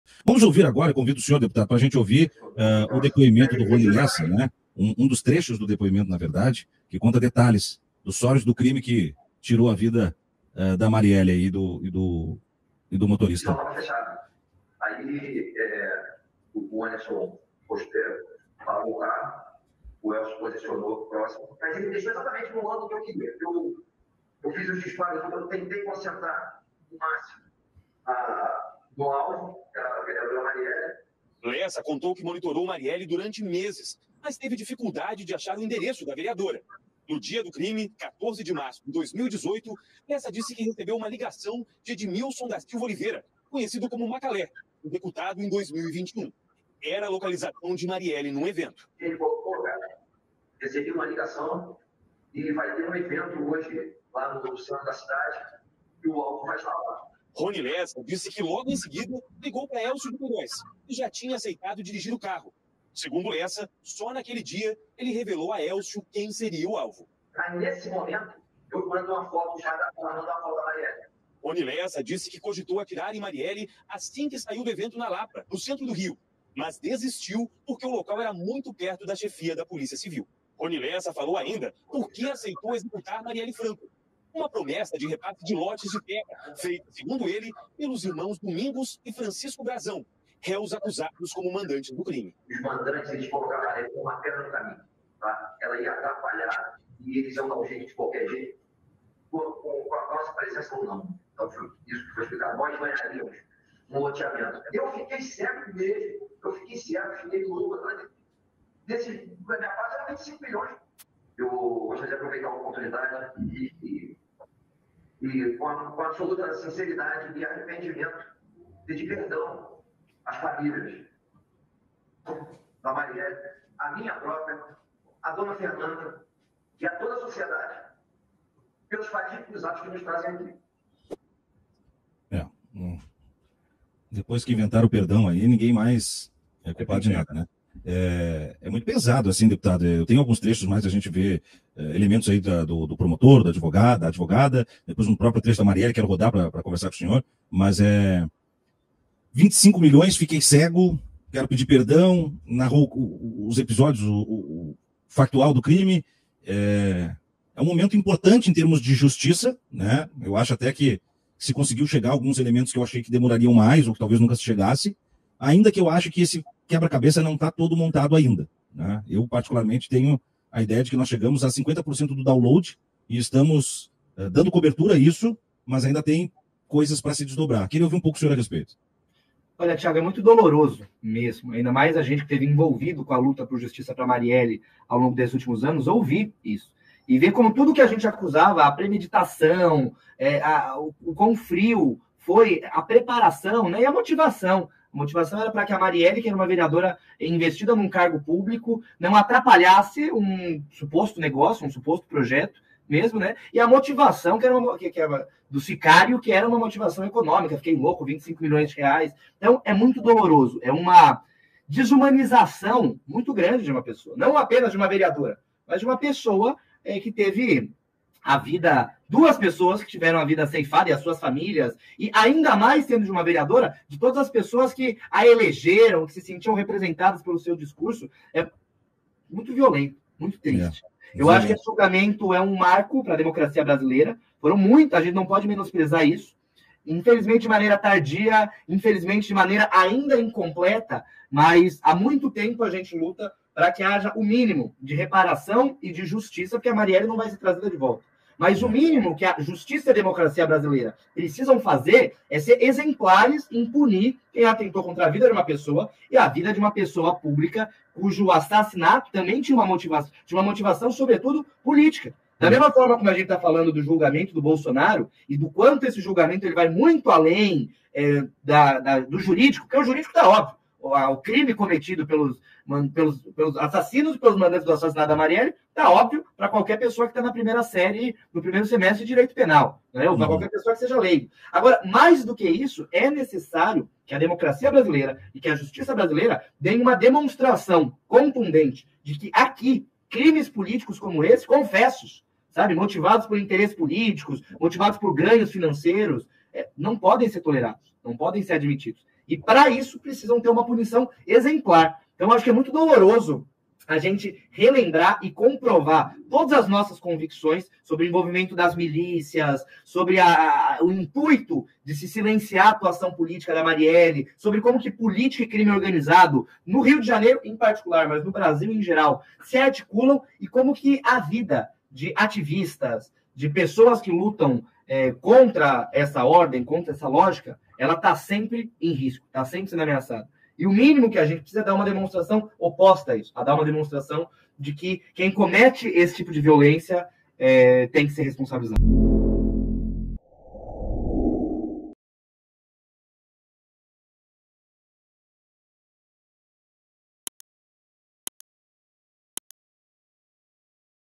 “Isso vai muito além do jurídico”, diz deputado do PSOL sobre julgamento dos assassinos de Marielle
Cortes DCM

🪙 envie contribuições
👥 seja membro